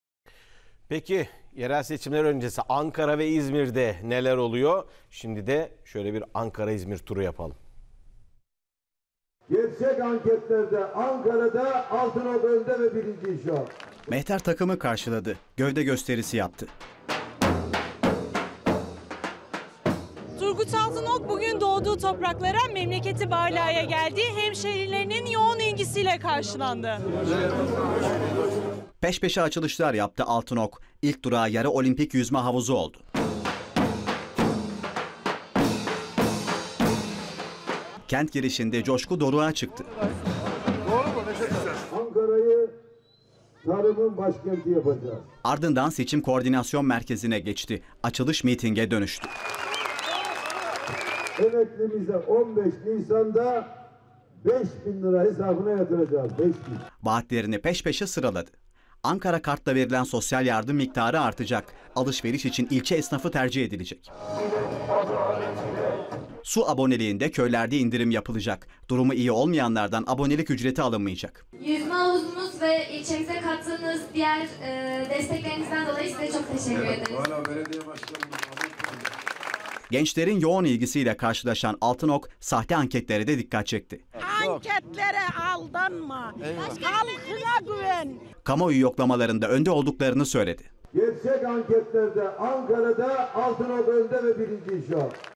Mansur Yavaş'a Yol Göründü! Turgut Altınok Anket Verilerini Paylaştı
AK Parti ve Cumhur İttifakı Ankara Adayı Turgut Altınok, anketler üzerinden manipülasyon yapıldığını ve son çalışmalarda önde olduğunu açıkladı.